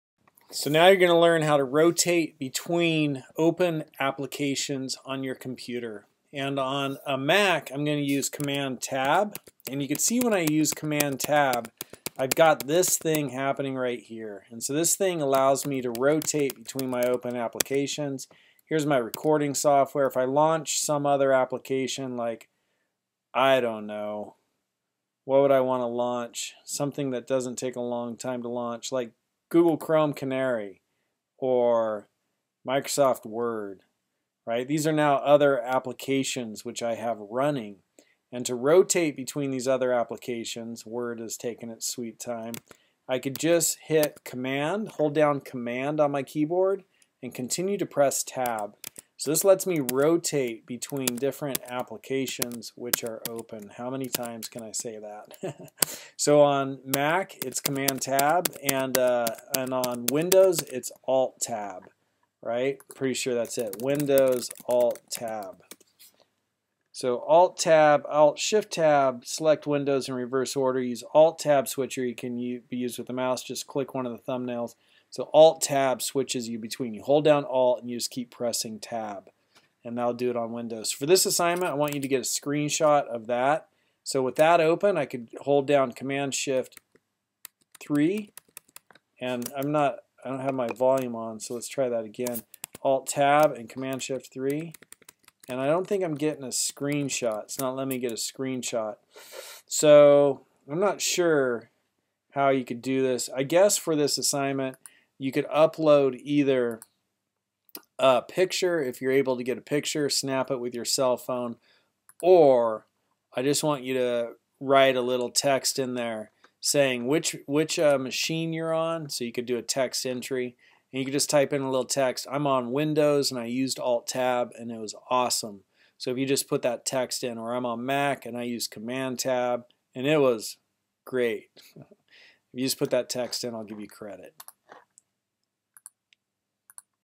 Keyboard shortcuts
Shortcuts when using computer. There are some minor differences between Windows and Mac.